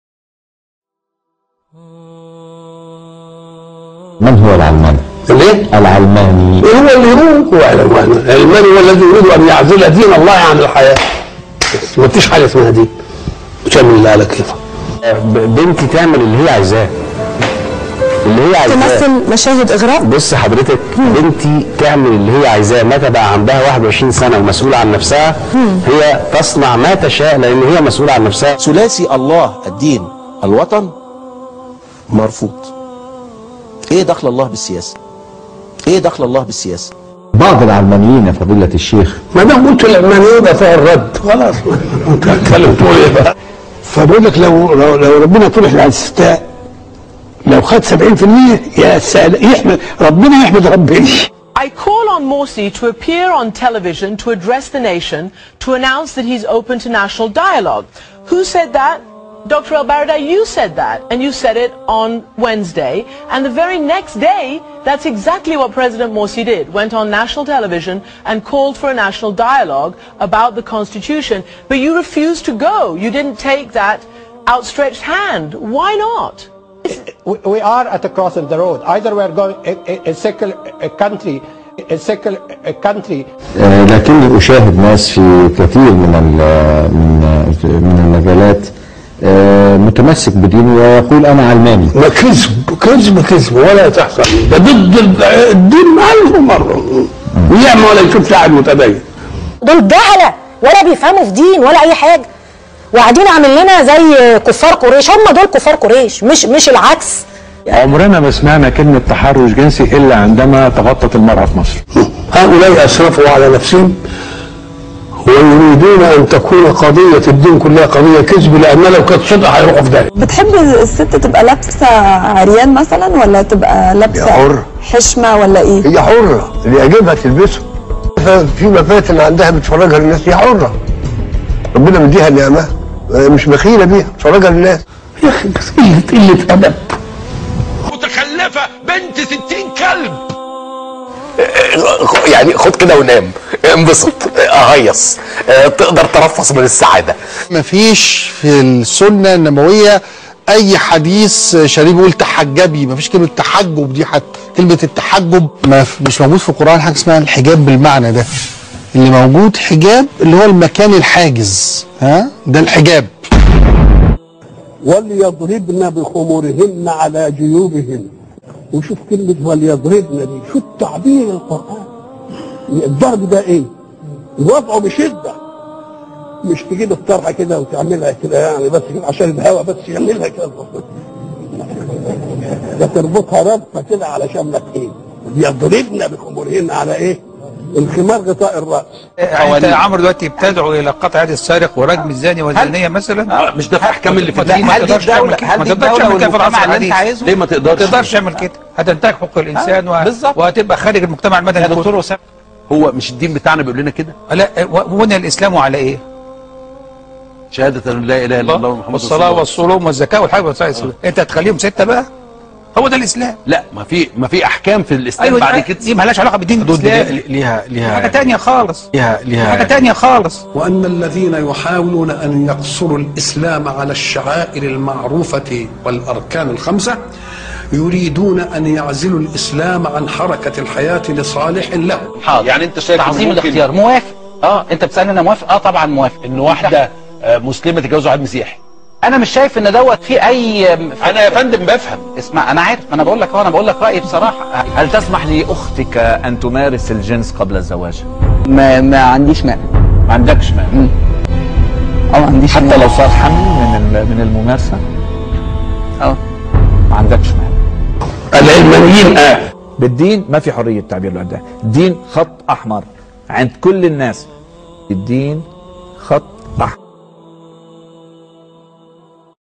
منهج العلمانين ... شاهد واحكم بنفسك
شاهد الفيديو لتتعرف بنفسك على مدى وضاعة الفكر العلمانيين .. ولتختار الى اى جهة تنحاز بفكرك